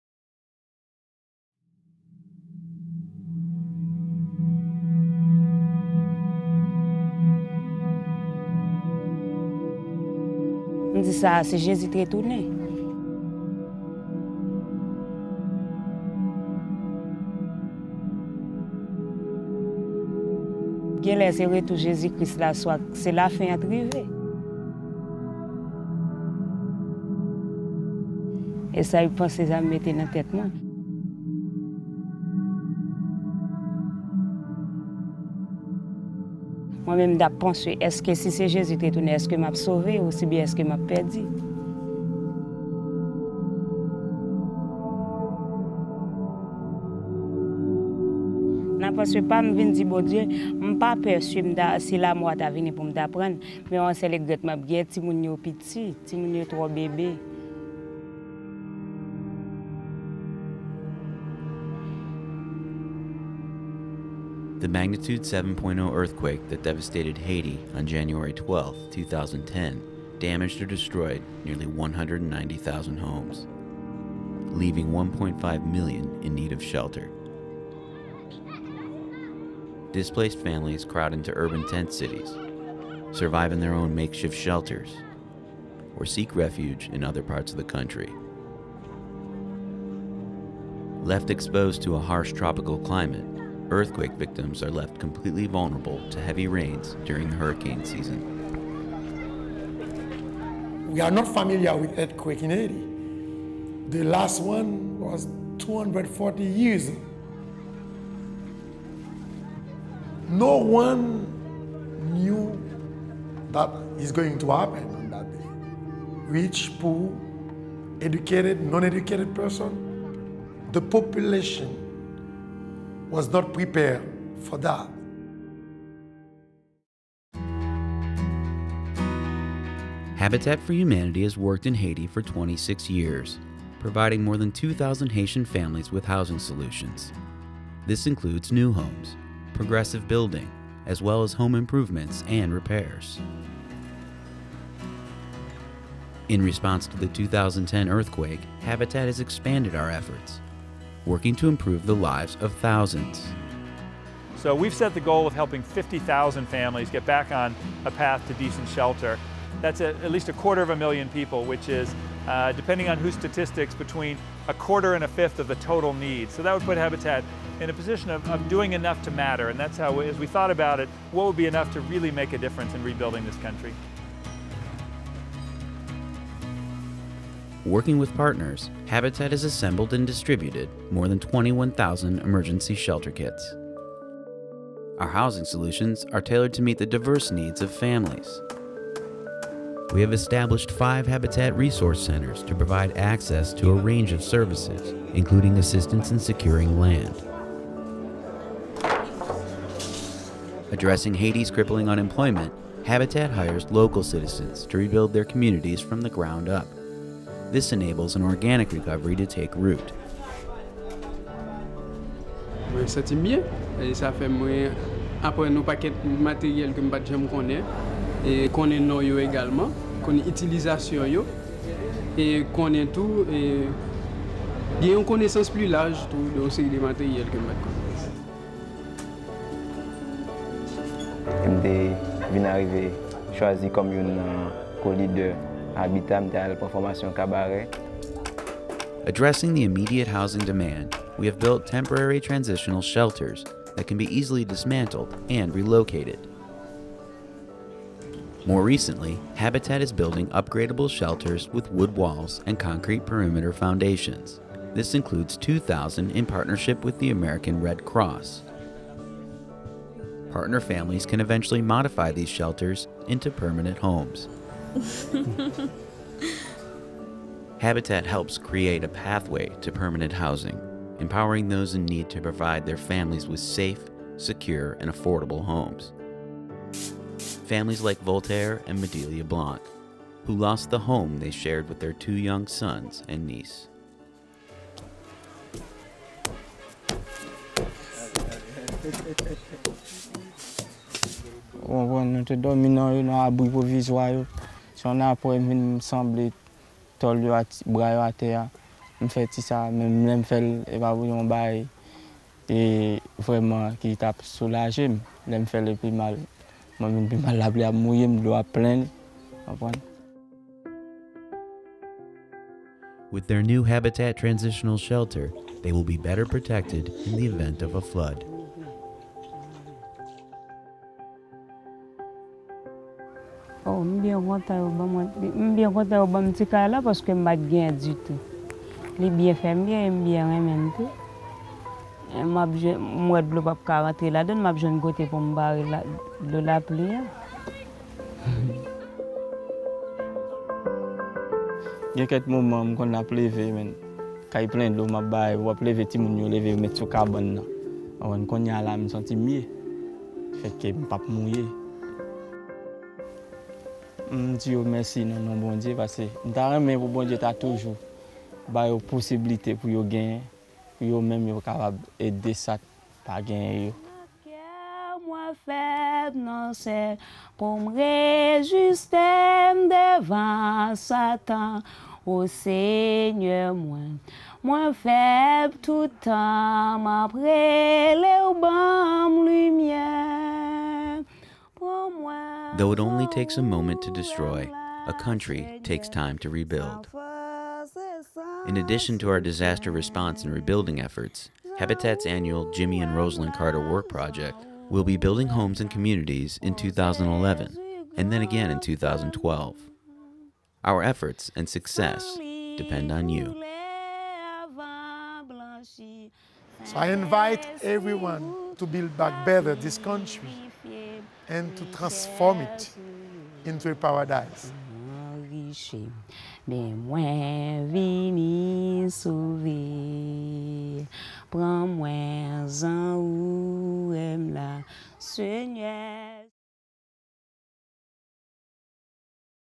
Building Hope in Haiti: One year later
Habitat has helped thousands of earthquake-affected families by distributing 21,000 emergency shelter kits and over 1,000 transitional or upgradeable shelters.